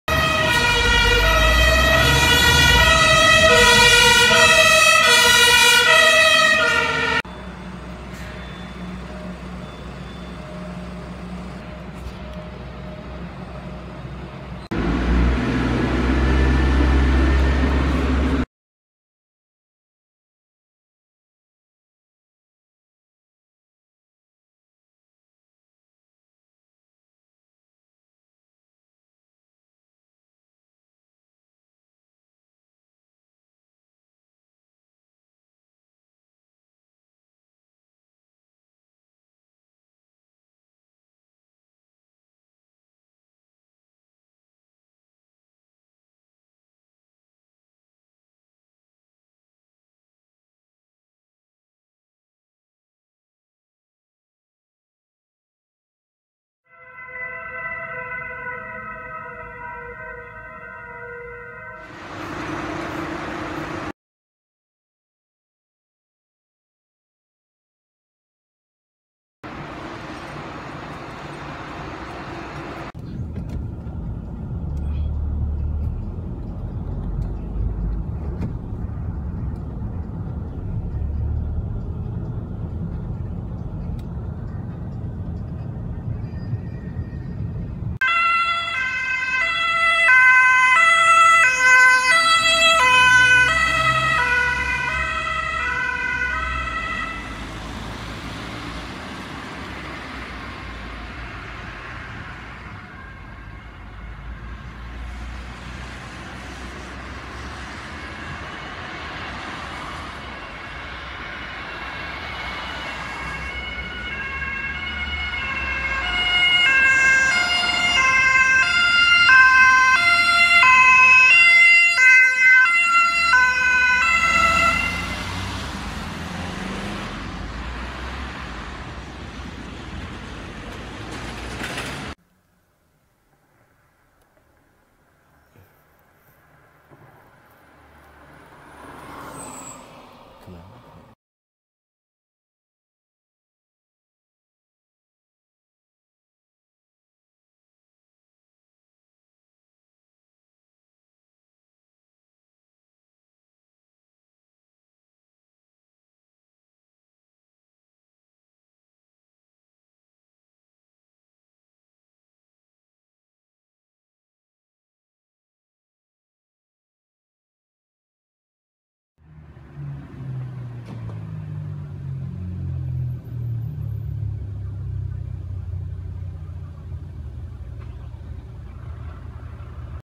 [ Werksfeuerwehr Berufsfeuerwehr Freiwillige Feuerwehr Rettungsdienst] Heilbronn Teil 1
Hier zu sehen der Zusammenschnitt von Heilbronn/ Landkreis